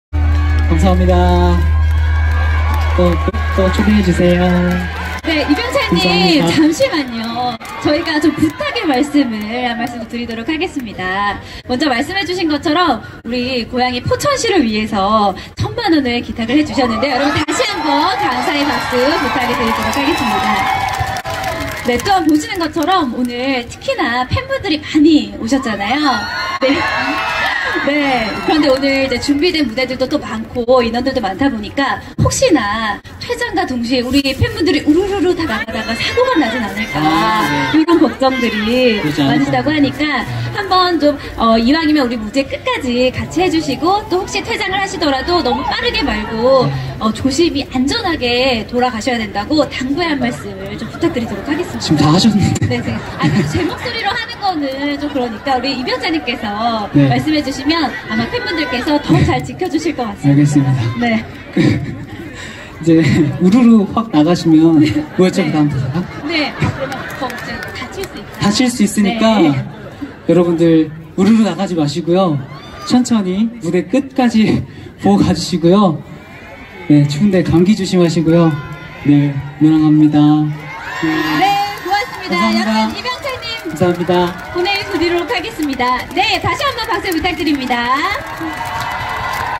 2022 제20회 포천시민의날/시민음악회/이병찬 천만원 기부알리는/유지은 아나운서
2022 제20회 포천시민의날 / 시민음악회 / 이병찬 천만원기부 알리는/
유지은 아나운서 그리고,첫무대라 4명의 가수들이 더 남아 있는 상황에서
팬들에게 끝까지 봐달라고 부탁하는 쎈쑤
그리고,말잘듣는 기부천사 이병찬과 팬들